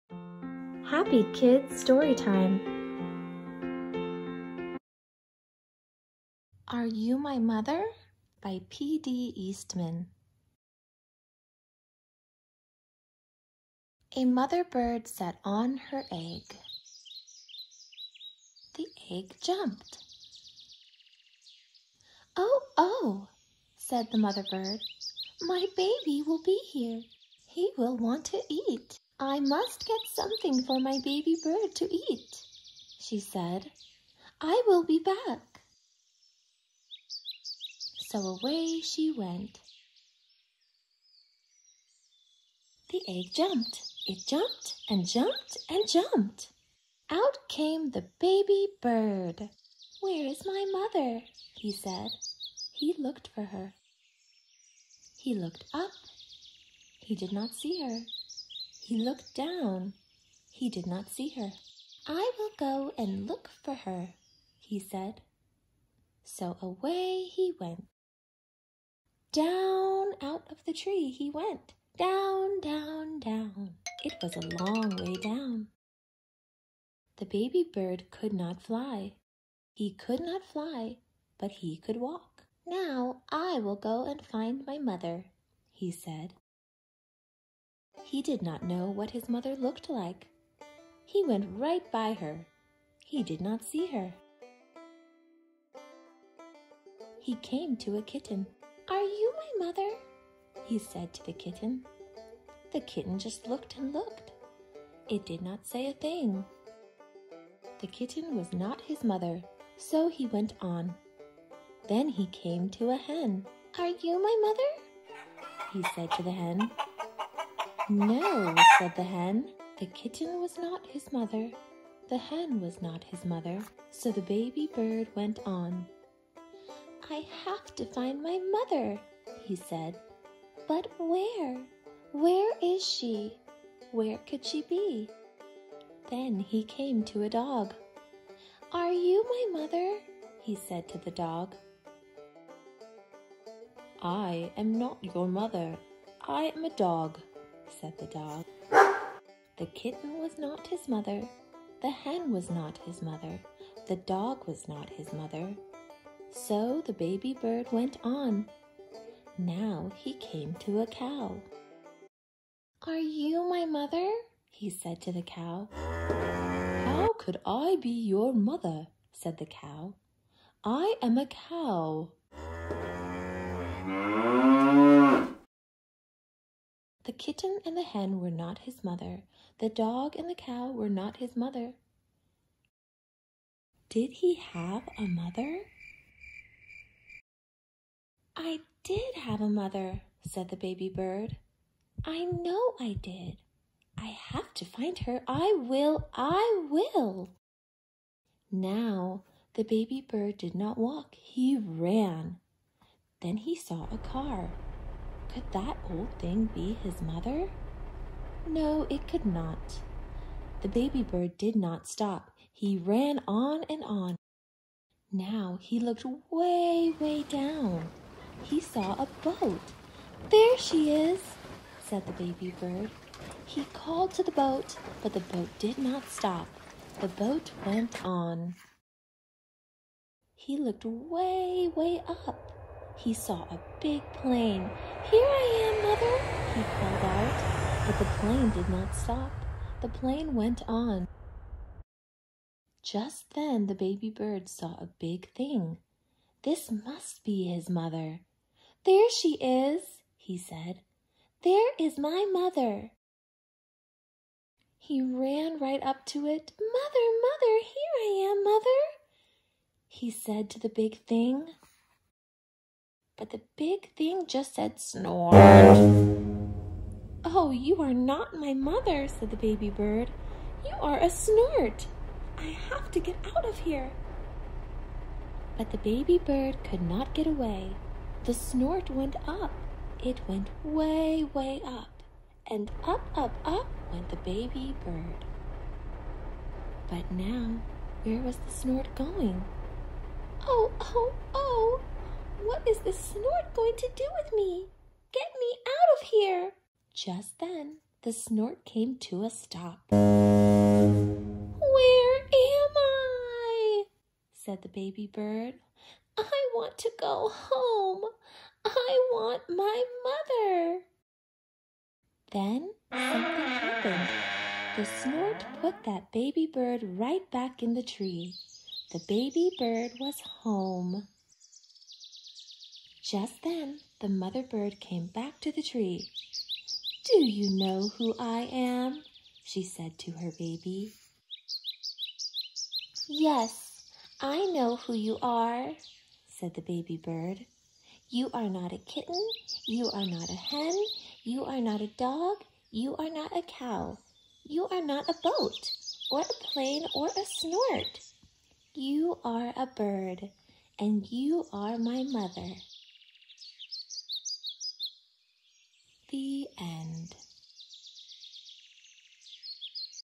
Happy Kids Storytime | Are You My Mother? By P.D. Eastman | READ ALOUD
Read aloud of one of the books that I remember loving to read as a child! The quest that this baby bird sets out on to find its' mother is full of bravery and determination. This little bird will not stop until it is safe back home in his mothers wings. Find out what he discovers and the animals and things he meets along the way! I hope you enjoy this story as much as I did as a kid! Love, Carmela

Voiceover: Carmela